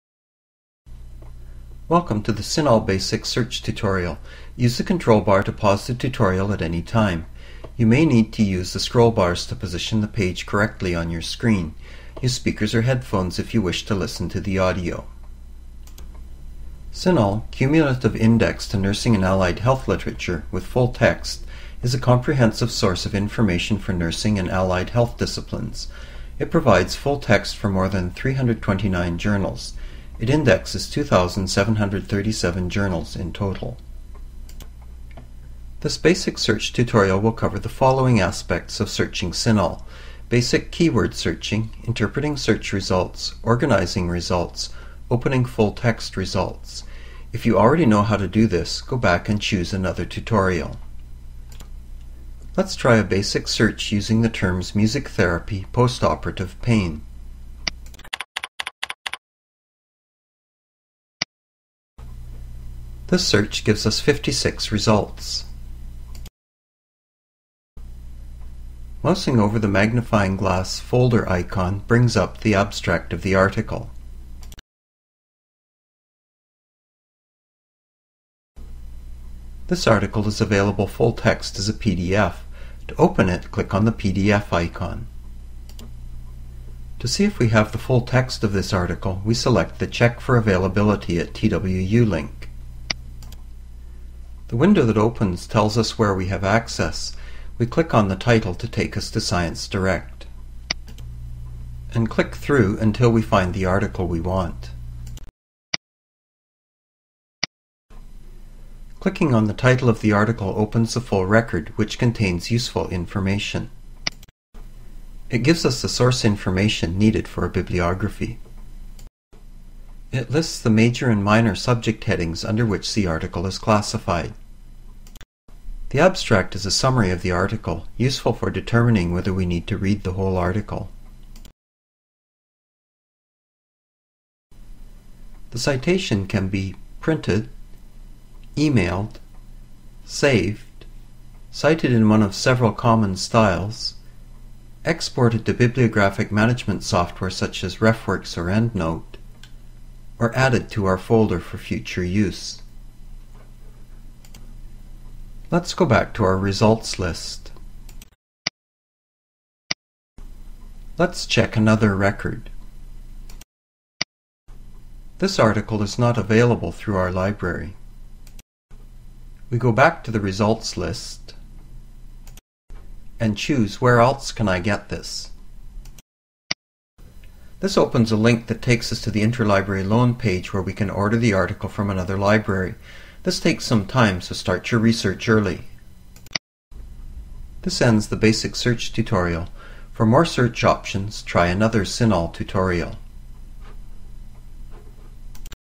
CINAHL EBSCO 2.0 Basic Search
A demonstration of the Basic search feature in the Cumulaitve Index to Nursing and Allied Health Literature (CINAHL) database. Covers basic keyword searching, interpreting search results, managing results, and retrieving full text articles. Created on Nov. 28, 2008 by Duncan Dixon using Adobe Captivate 3. Approximately 3:16 minutes. The Adobe Captivate 3 source file and .swf file for this tutorial are available from the ANTS repository at the University of Calgary